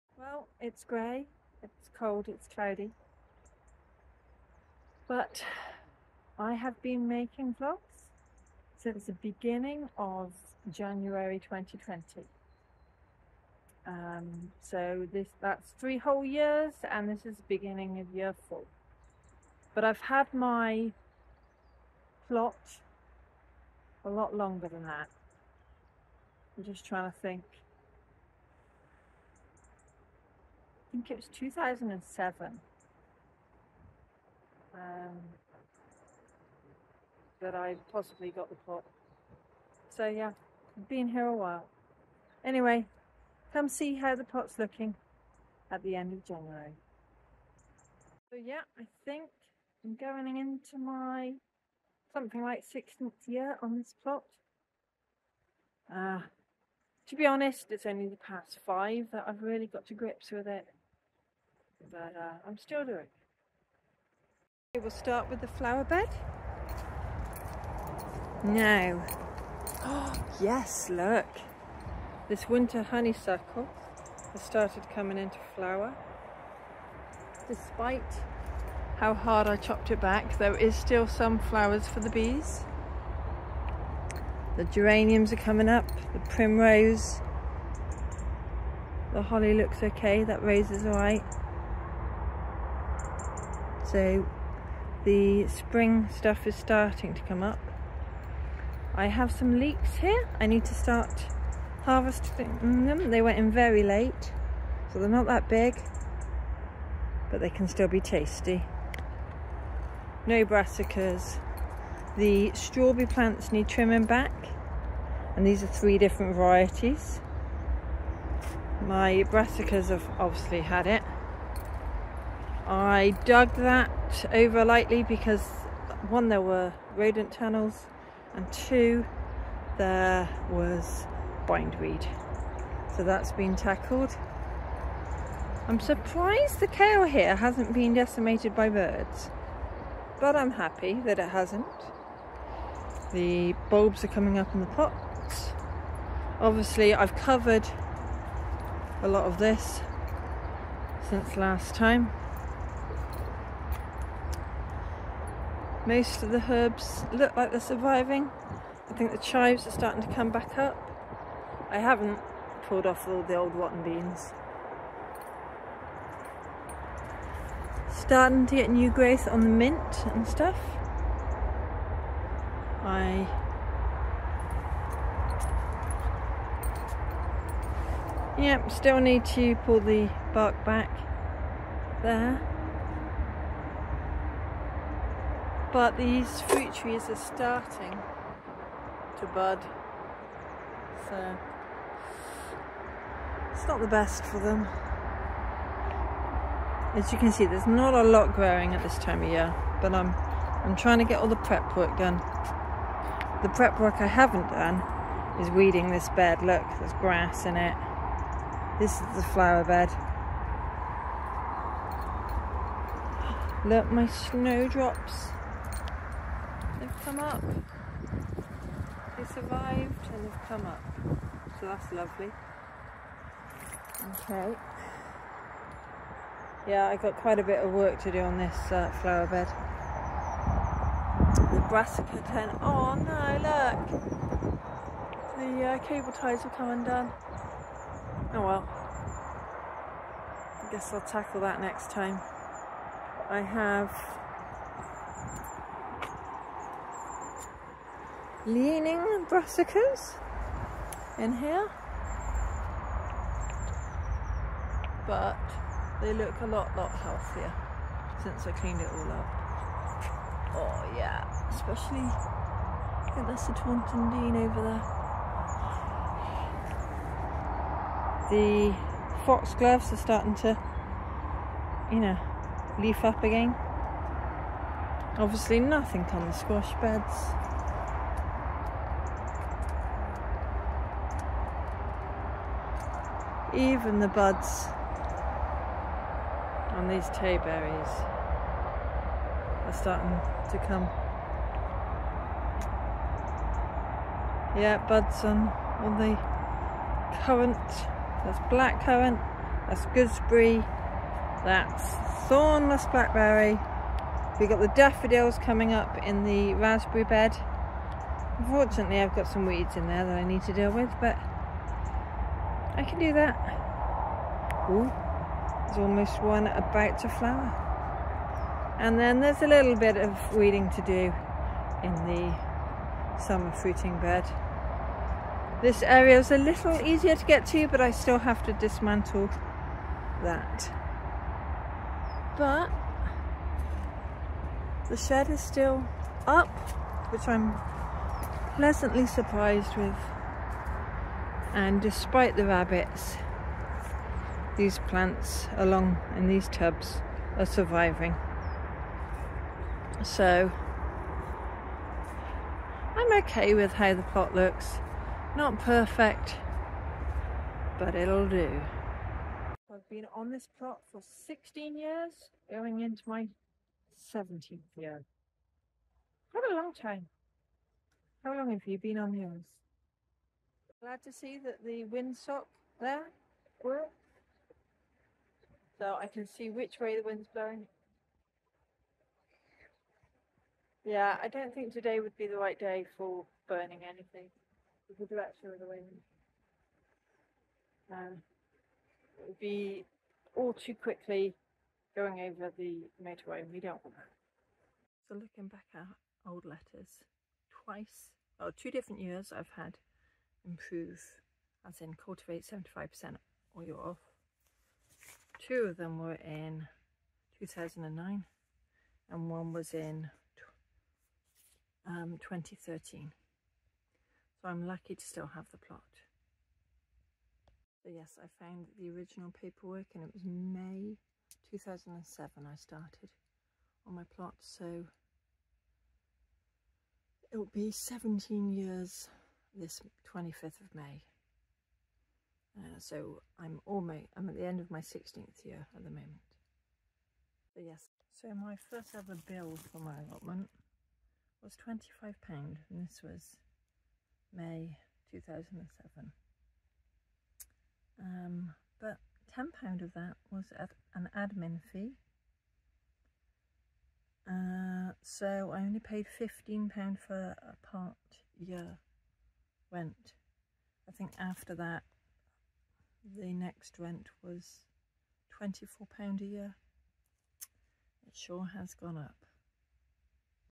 January tour & 3 years on you tube
3 whole years on YouTube and a quick look at the plot